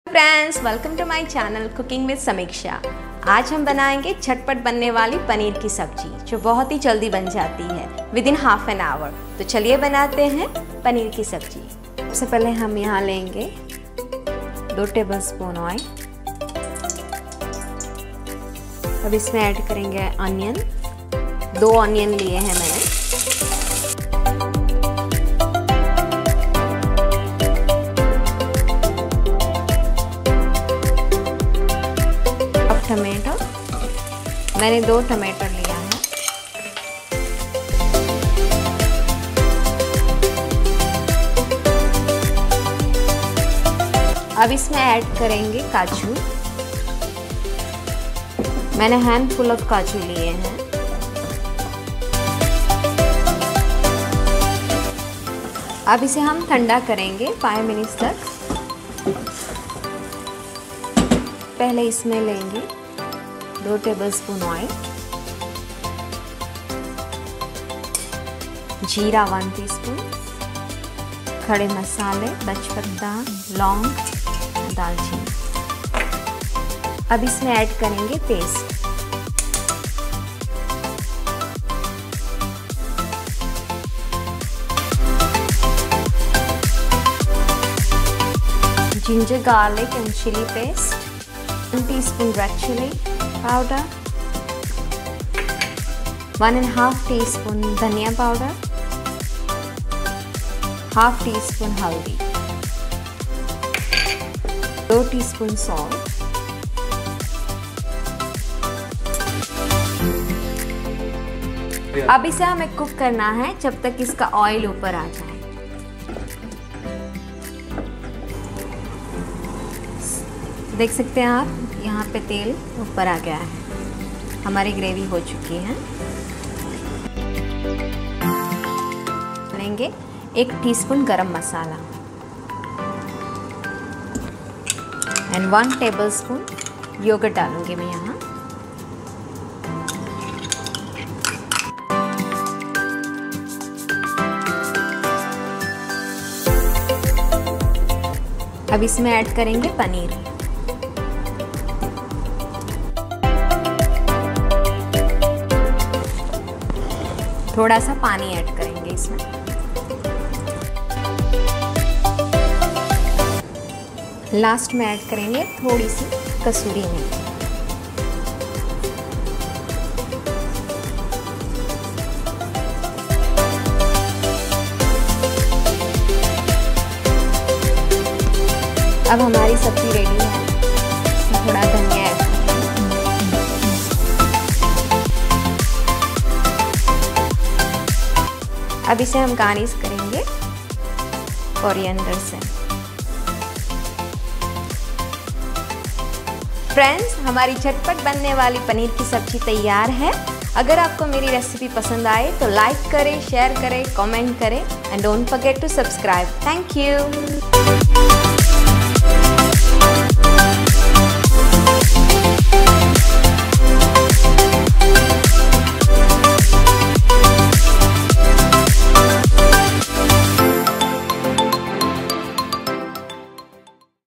Paneer Sabji (quick & easy) || पनीर सब्ज़ी
Today we will be cooking Paneer ki Sabji quickly and easily with minimal ingredients.